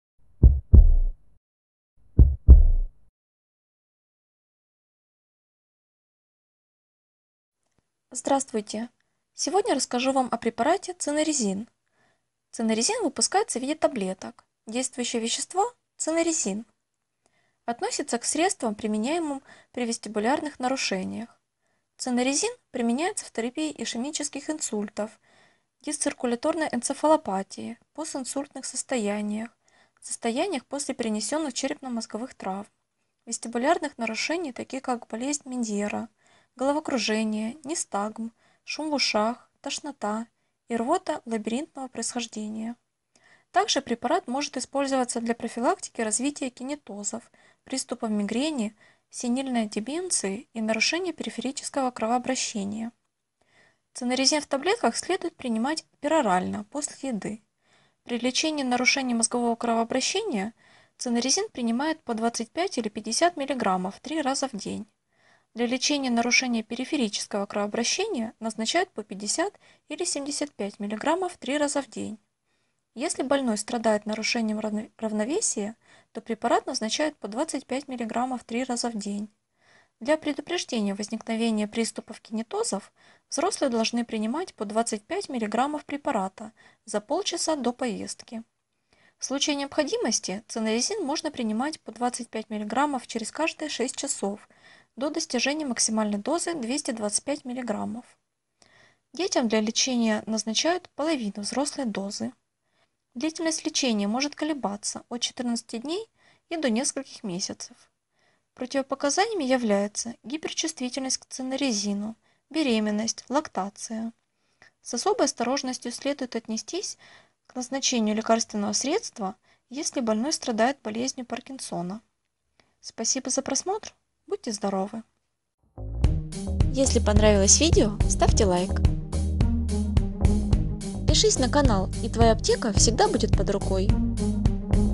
Циннаризин таблетки - показания (видео инструкция) описание, отзывы
Показания
Циннаризин Софарма назначают:

- для лечения расстройств цереброваскулярного происхождения (шум в ушах, головокружение, раздражительность, снижение памяти, концентрации внимания, головная_боль боль);

- при вестибулярных симптомах (болезнь Меньера, шум в ушах, головокружение, нистагм, тошнота лабиринтного происхождения);

- для предупреждения "дорожной болезни" (кинетозов), таких как морская и воздушная болезни;

- для предупреждения приступов мигрени;

- при нарушениях периферического кровообращения (тромбофлебит, "перемежающаяся" хромота, парестезии, диабетическая ангиопатия, акроцианоз, ночные спазмы, похолодание в конечностях, болезнь Рейно);

- при трофических нарушениях (трофические язвы, варикозные язвы).

Противопоказания
Циннаризин Софарма нельзя назначать:

- при аллергии на компоненты;

- при кормлении грудью;

- детям до 5 лет.

С осторожностью Циннаризин Софарма применяется при болезни Паркинсона.

Препарат Циннаризин Софарма может быть назначен при необходимости.

Нет данных о проникновении циннаризина в молоко матери, поэтому кормление грудью прекращают.

Внимание!